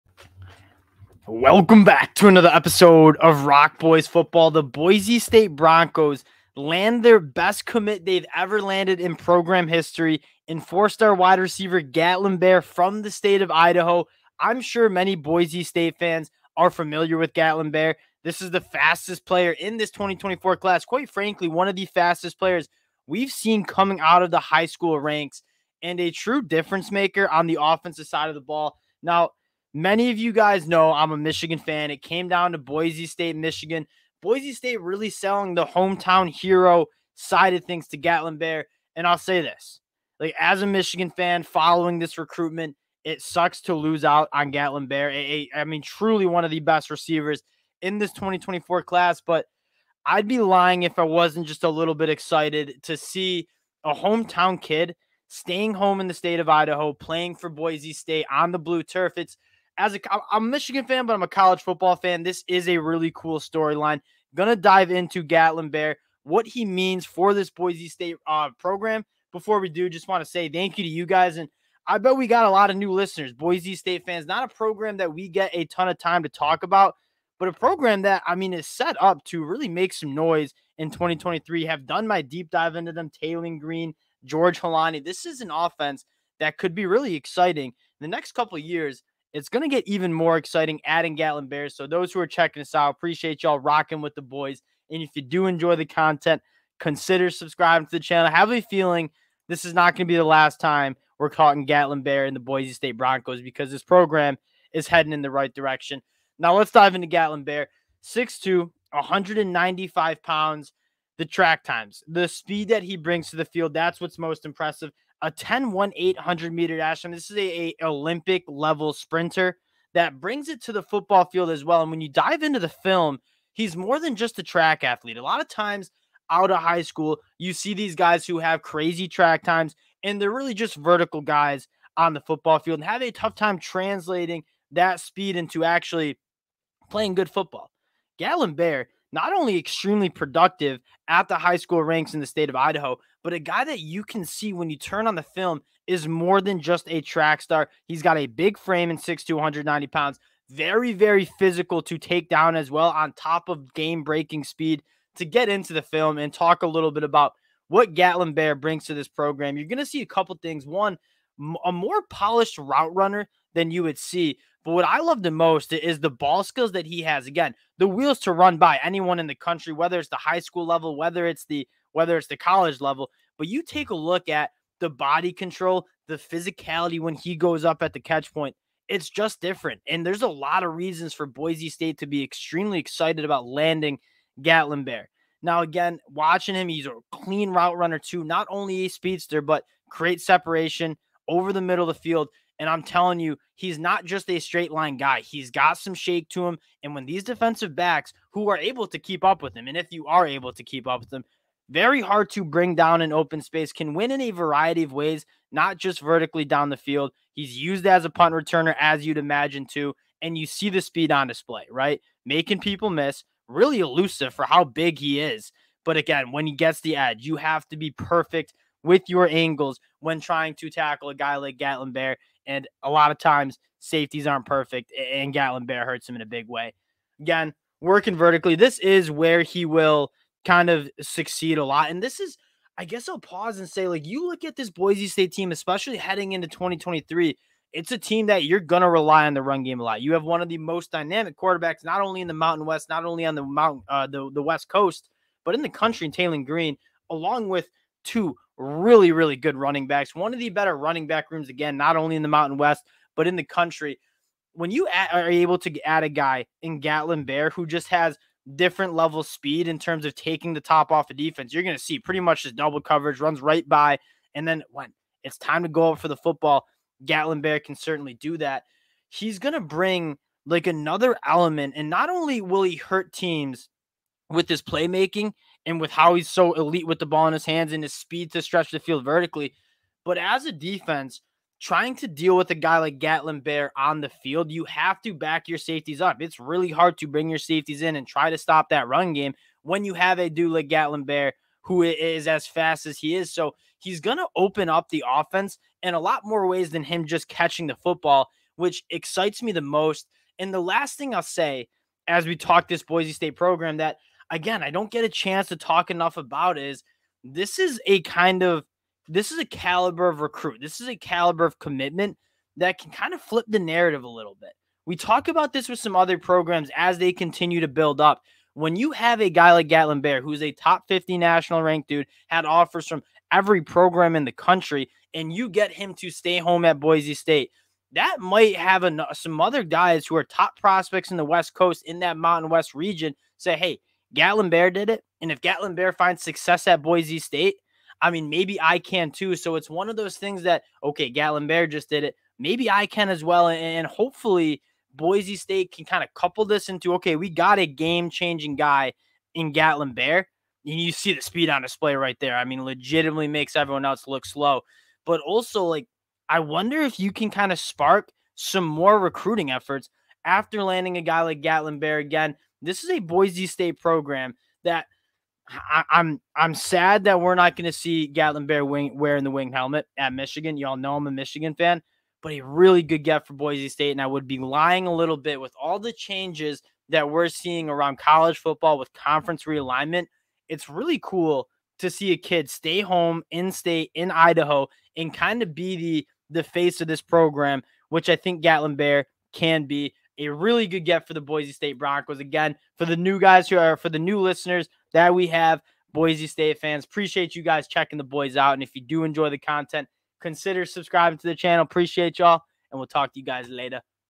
Boise State Football Lands the BEST Commit in Program History | Gatlin Bair Commits to the Broncos
The Boise State Broncos land the best commit in program history as 4 star WR Gatlin Bair. The fastest player in the 2024 cycle posts a 10.18 in the 100 meter.